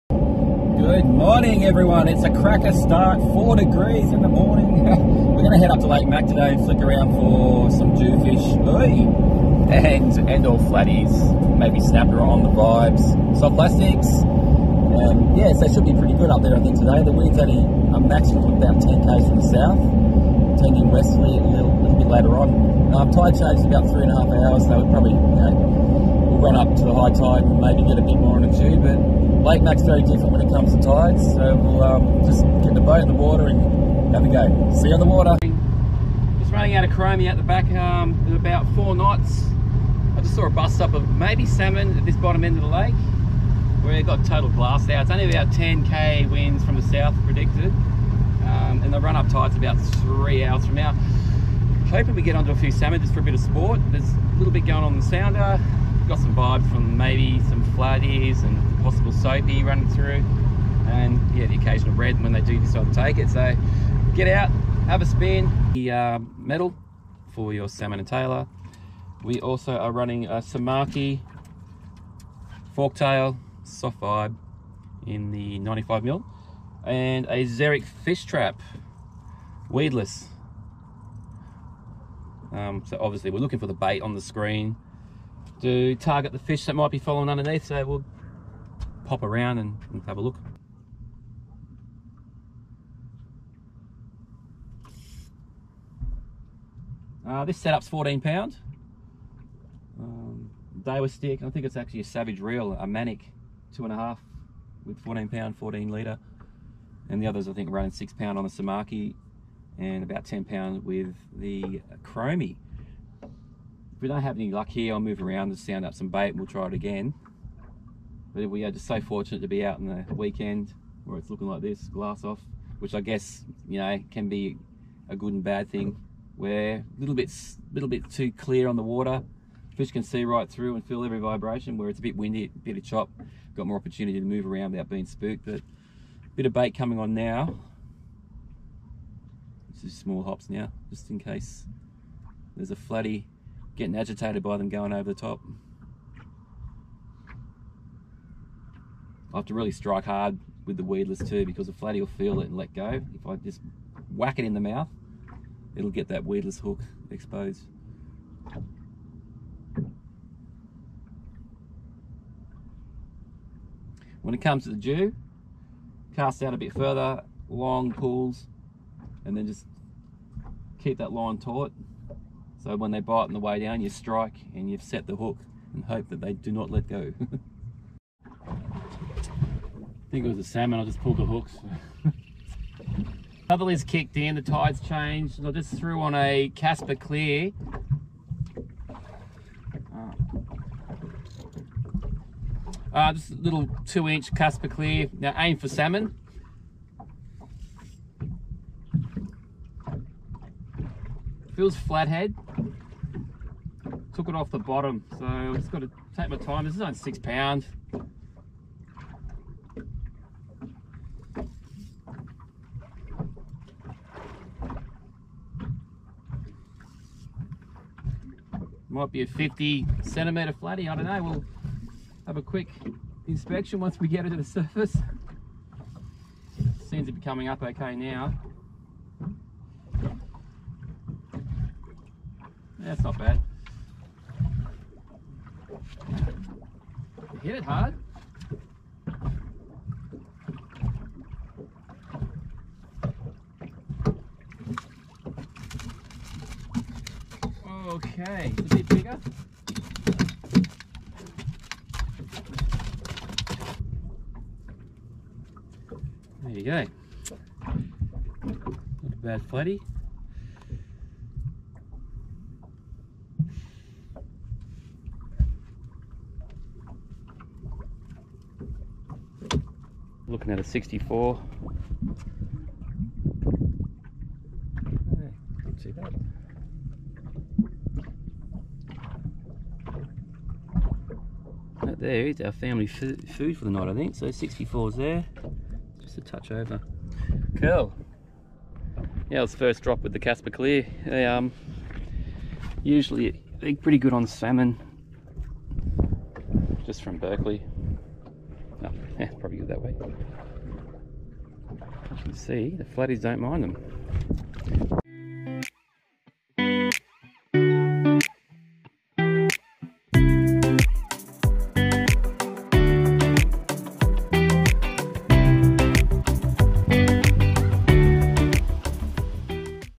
Quick tech talk on lure methods for 1 fish 😂
Well, with the calm weather the bites tend to drop off as the fish can see what's happening and feel what's happening a lot easier . It's only when the wind picks up, I start to catch something.